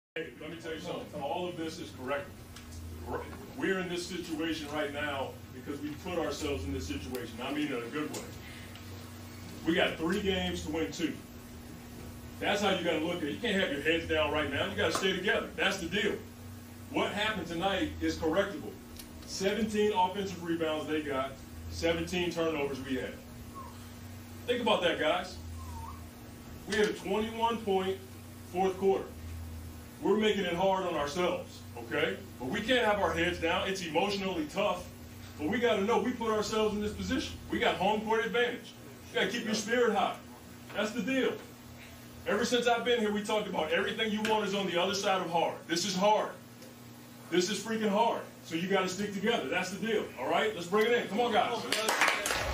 Monty Williams' message to Devin Booker and the Suns
Follow my new media channel Monty Williams talks to his team after losing Game 4 | Bucks vs Suns Game 4